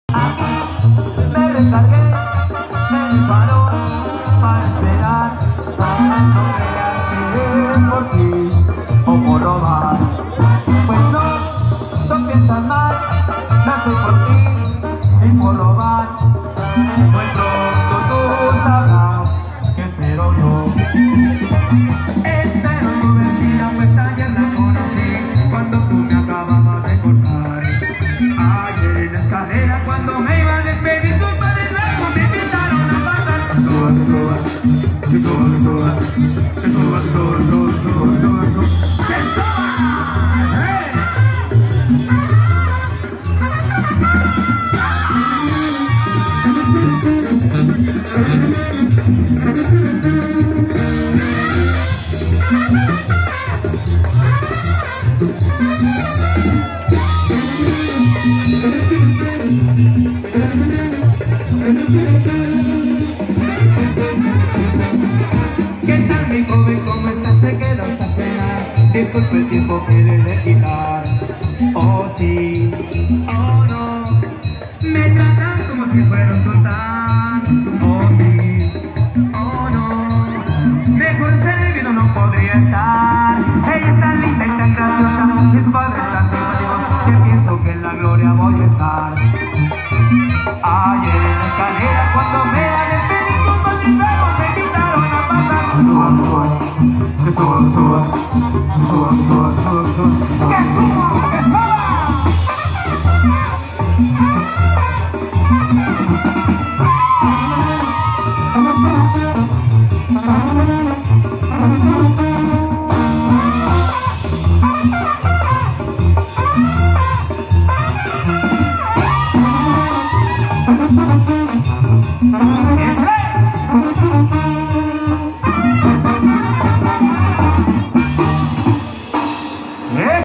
Nico Flores. Lienzo Charro El Recodo del 40 Valp. Zac. 24 de dic 22. (Tradicional Coleadero )
Tardeada baile con Nico Flores y los Originales de la Banda.